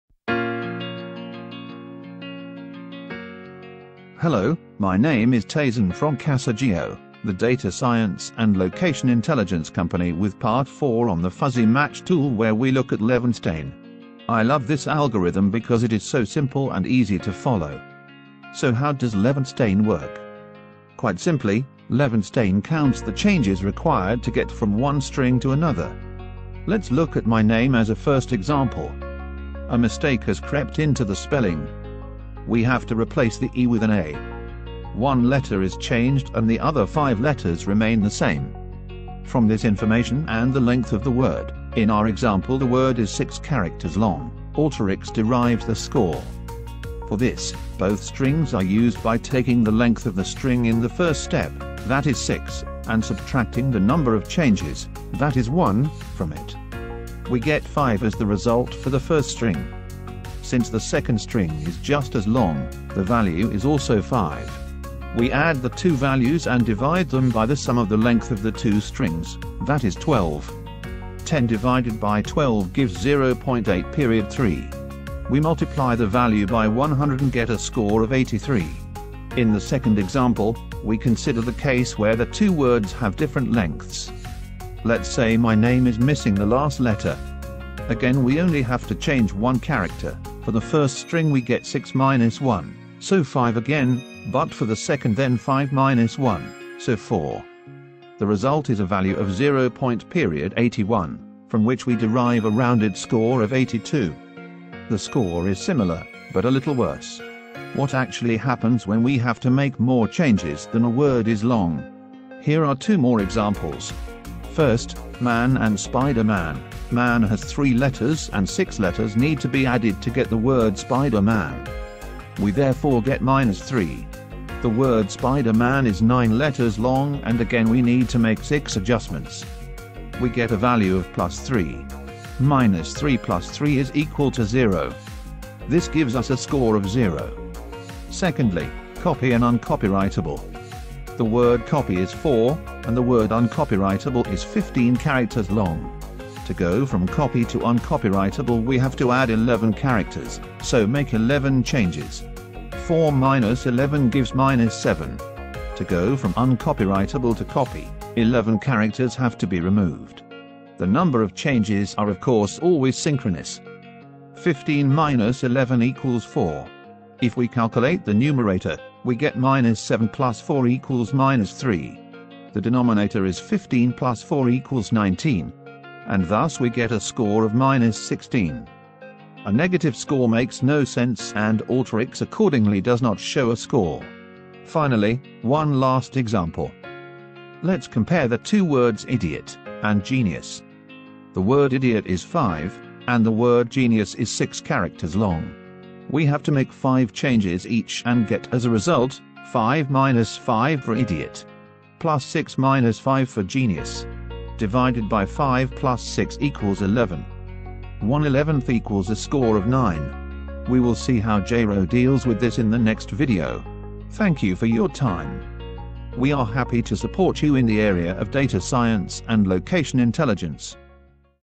Fuzzy Match Tool - #04 Levenshtein Distance
In this video, we show examples of how the score for the Levenshtein Distance is calculated in the Fuzzy Match Tool in Alteryx.